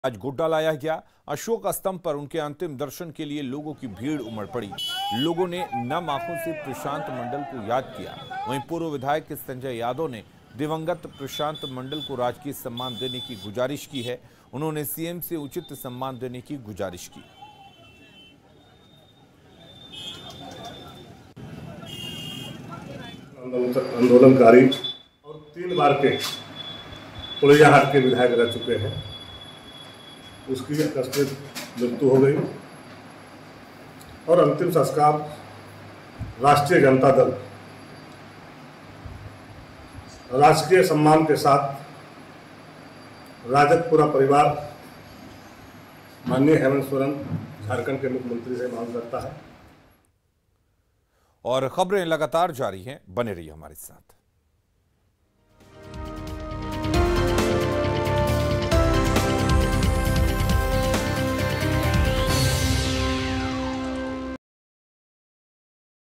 Godda से RJD के पूर्व विधायक Sanjay Yadav की मांग, Prashant Mandal को मिले राजकीय सम्मान
Prashant Mandal के पार्थिव शारीर को आज godda लाया गया. इस दौरान भारी संख्या में भीड़ उमड़ पड़ी, लोगों ने नाम आँखों से Prashant Mandal को याद किया, वहीँ Godda से RJD के पूर्व विधायक Sanjay Yadav ने मांग की है कि Prashant Mandal को राजकीय सम्मान मिले.